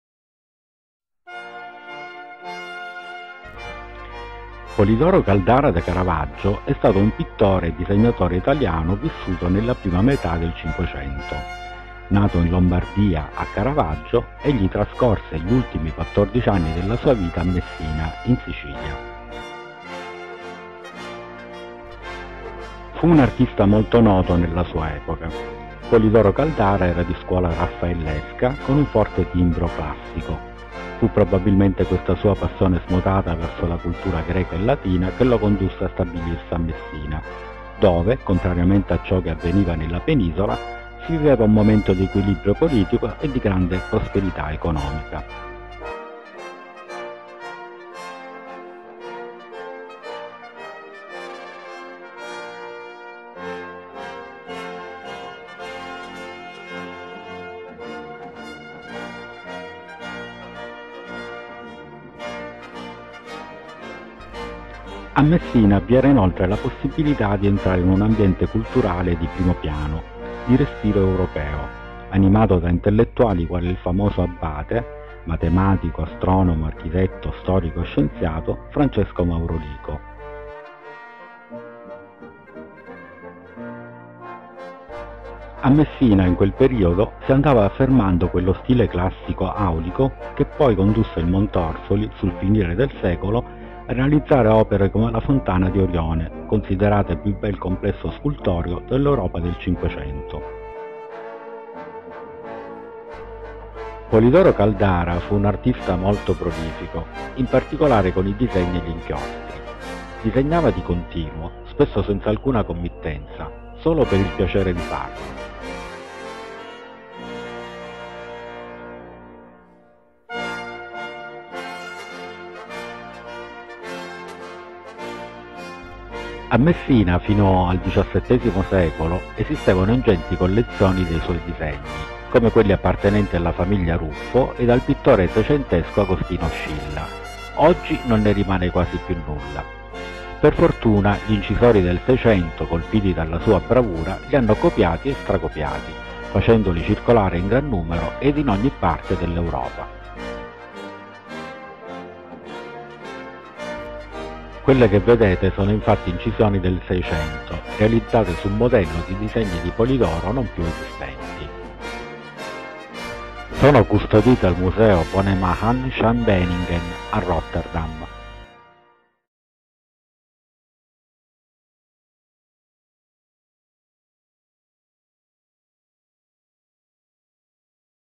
Polidoro Caldara da Caravaggio. Il mitico '500 di Messina. by LuLoCa
Polidoro Caldara. The legendary '500 of Messina. by LuLoCa

Polidoro Caldara da Caravaggio was an Italian painter and illustrator living in the first half of the sixteenth century. Born in Lombardy, Caravaggio, he spent the last 14 years of his life in Messina, Sicily.
 It was a well-known artist in his day. Polidoro Caldara was the school of Raphael, with a strong classical tone. It was probably this immoderate passion towards Greek culture led him to settle in Messina, where, contrary to what was happening on the peninsula, is living a moment of political stability and great economic prosperity. In Messina, there was also the possibility of entering into a cultural environment of the first floor, with a European, led by intellectuals such as the famous Abbot mathematician, astronomer, architect, historian and scientist Francis Maurolico, profound knowledge of classical culture. In Messina, at the time, was developing the noble classical style which then led Montorsoli, at the end of the century, to produce works such as the fountain of Orion, considered the finest of sculptures of Europe's 500.
 Polidoro Caldara was a very prolific artist: especially with the drawings and inks. He drew constantly, often without any commission, just for the fun of it. In Messina, until the seventeenth century there were large collections of his drawings, like those belonging to the family Ruffo and the seventeenth-century painter Agostino Scilla. Today almost nothing remains. Fortunately, the engravers of the '600, impressed with his skill, have them copied and stracopiati, causing them to circulate in large numbers and in every part of Europe. What you see are in fact recordings of 600, made ​​on a form of drawings by Polidoro no longer extant, preserved at the museum in Rotterdam Beninghen Bonemahn shan.

Polidoro Caldara da Caravaggio è stato un pittore e disegnatore italiano vissuto nella prima metà del Cinquecento. Nato in Lombardia, a Caravaggio, egli trascorse gli ultimi 14 anni della sua vita a Messina, in Sicilia.
Fu un artista molto noto nella sua epoca. Polidoro Caldara era di scuola raffaellesca, con un forte timbro classico. Fu probabilmente questa sua passione smodata verso la cultura greca e latina che lo condusse a stabilirsi a Messina, dove, contrariamente a ciò che avveniva nella penisola, si viveva un momento di equilibrio politico e di grande prosperità economica. A Messina, vi era inoltre la possibilità di entrare in un ambiente culturale di primo piano, di respiro europeo, animato da intellettuali quali il famoso abate matematico, astronomo, architetto, storico e scienziato Francesco Maurolico, profondo conoscitore della cultura classica. A Messina, in quel periodo, si andava affermando quello stile classico aulico che poi condusse il Montorsoli, sul finire del secolo, a realizzare opere come la fontana di Orione, considerata il più bel complesso scultoreo dell'europa del 500.
Polidoro Caldara fu un artista molto prolifico: in particolare con i disegni e gli inchiostri. Disegnava di continuo, spesso senza alcuna committenza, solo per il piacere di farlo. A Messina, fino al diciassettesimo secolo esistevano ingenti collezioni dei suoi disegni, come quelli appartenenti alla famiglia Ruffo ed al pittore seicentesco Agostino Scilla. Oggi non ne rimane quasi più nulla. Per fortuna, gli incisori del '600, colpiti dalla sua bravura, li hanno copiati e stracopiati, facendoli circolare in gran numero ed in ogni parte dell'Europa. Quelle che vedete sono infatti incisioni del 600, realizzate su modello di disegni di Polidoro non più esistenti, custodite al museo Bonemahn shan Beninghen a Rotterdam.